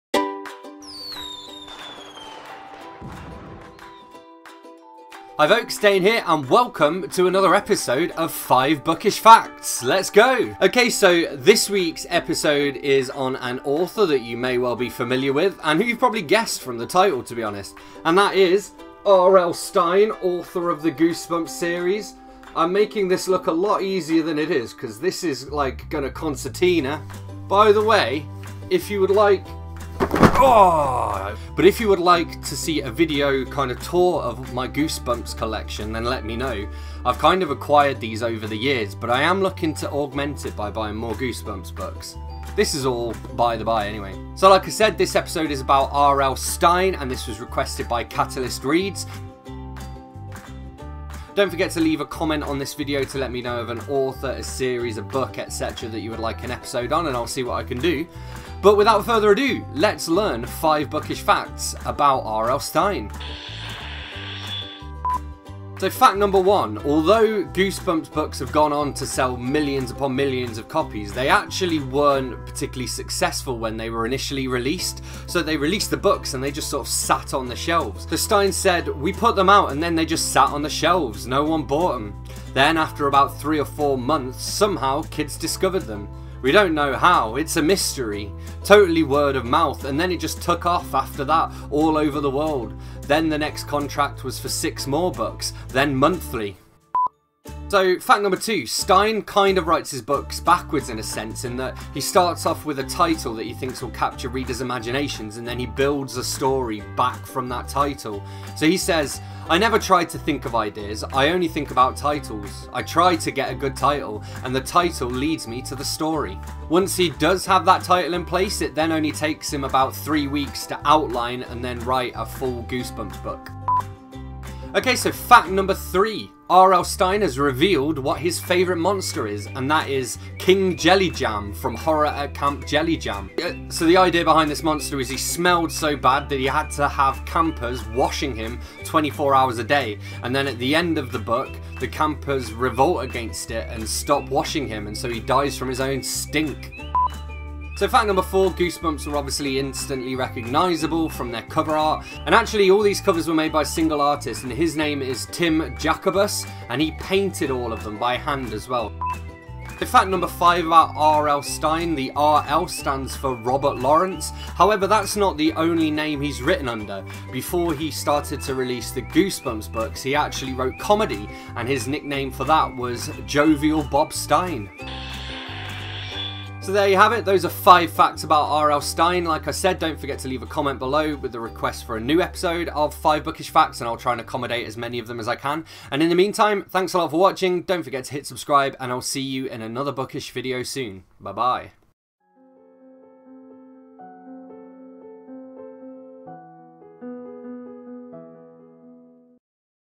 R. L. Stine [FIVE BOOKISH FACTS]
It's Wednesday, which means it's time for a new episode of Five Bookish Facts! Thanks to Catalyst Reads for requesting this one. Got a request for a future episode of Five Bookish Facts? Let me know with a comment!

YOUTUBERS MENTIONED:
Catalyst Reads